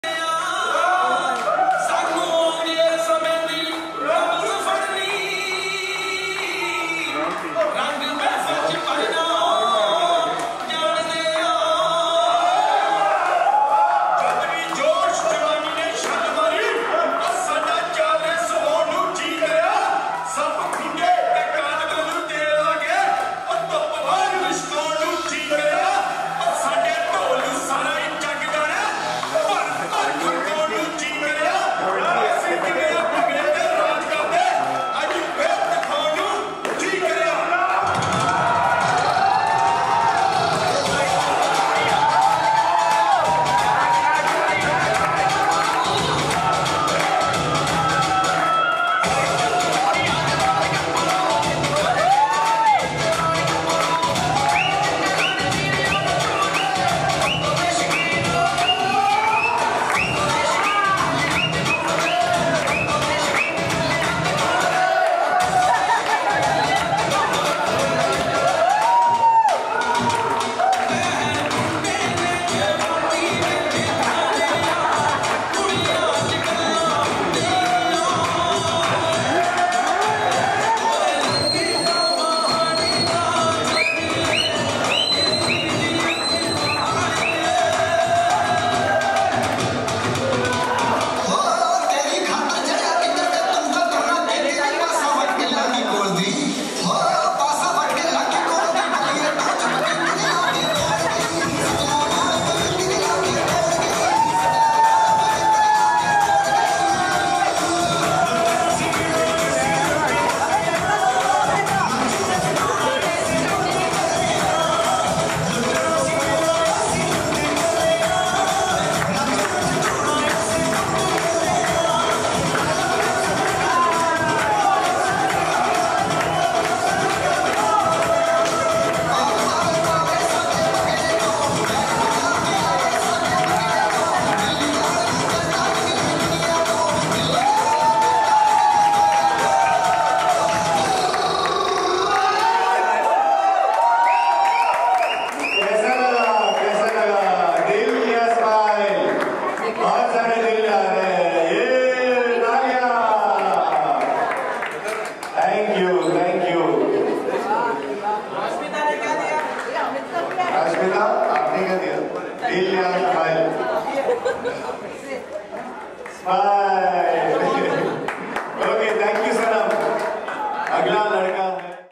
Peter England Mr India 2017 Sanam Bariar's Dance Performance
Watch Peter England Mr India 2017 Sanam Bariar's Dance Performance.

Co Powered by Zevenworld

Venue: JJ Dharamshala, Byculla